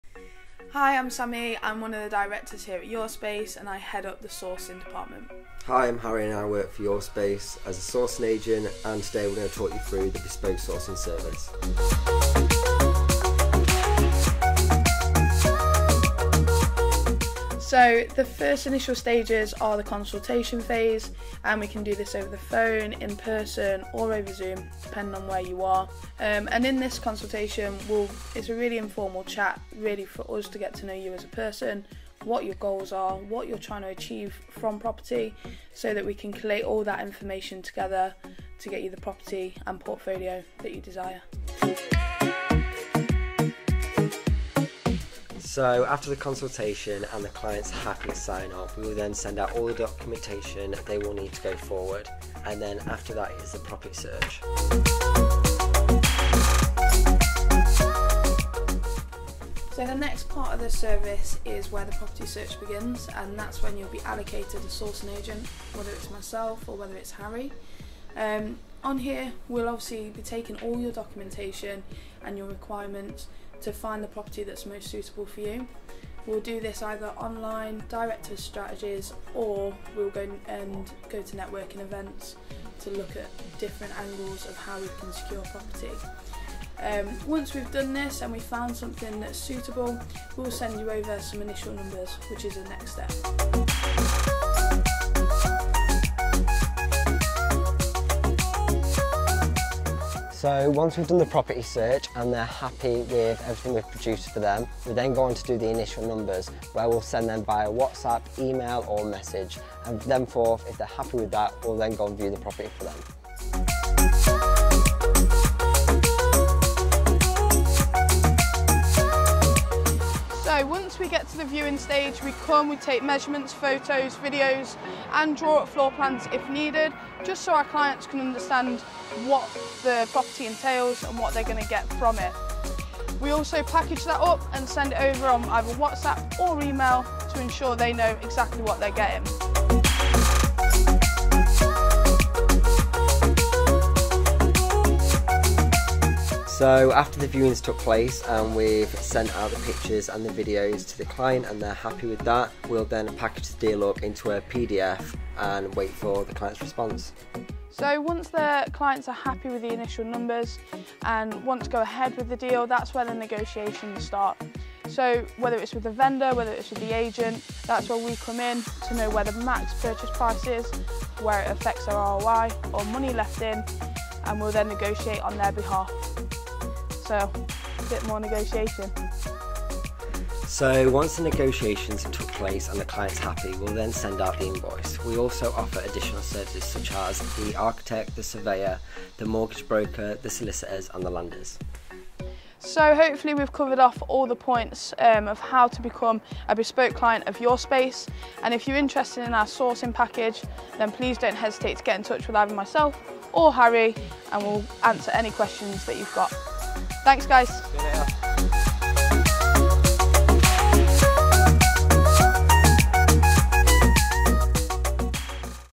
Bespoke sourcing | Services
BESPOKE PROPERTY SOURCING SERVICES
We pride ourselves in providing a service tailored to meet the specific needs of property investors.
By creating a legal, bespoke service it allows us to understand our clients and their property investment needs when securing a property.

This makes us unique as we only view properties once we get the go ahead from the investor, meaning no time is wasted within the property search.

STEPS TO OUR SOURCING SERVICE
1) Sign up to bespoke sourcing service
An initial consultation, either on the phone or via Zoom, talking strategies, goals and what you are looking for. Followed-up with us sending you your personalised client information pack.

2) Property search
During this time we will be viewing properties, on your behalf; carrying out due diligence; presenting/packaging deals; presenting budget build costs, relating to the property.

3) Deal acceptance
Once you are happy with a deal we will begin the negotiations and secure the property purchase. After the purchase price is agreed, you will have several actions to carry out. Once the conveyancing period is complete, the property will complete and move into the design & phase.

4) Deal feasibility
If you are looking to take on our construction design service, this starts with our sourcing team, working with our architect to complete the initial feasibility on the project. Which includes a project scope of works; building survey; site visit, with the architect; site measured survey; completion of existing drawings and proposed sketches.

Extra support
Additional support service
If you are new to property investing, or want to be more hands-off during the purchasing process, we offer an extra support package that uses our mortgage broker, solicitors, surveyors and design & build team. We can support you with the purchase, by liaising with the required parties throughout, to ensure that your deal goes through smoothly.

Interested in our investment or design and build services?
Call the office on:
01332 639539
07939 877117